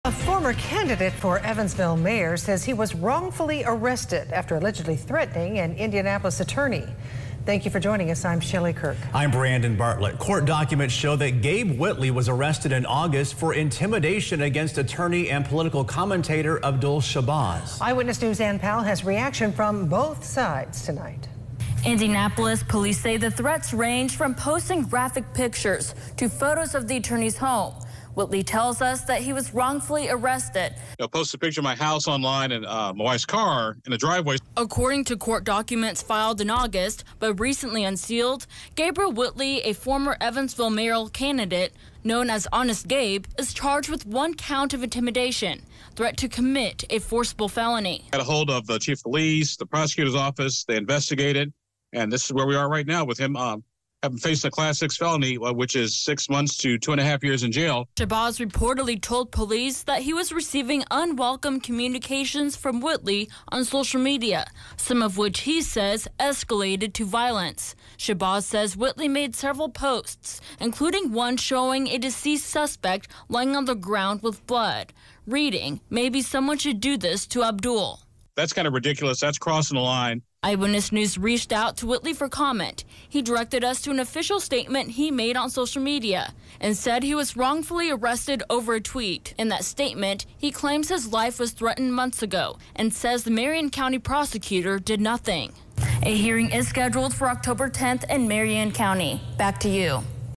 Gabe Whitley arrested in August for intimidation
A former Evansville mayoral candidate has been charged after allegedly threatening an Indianapolis attorney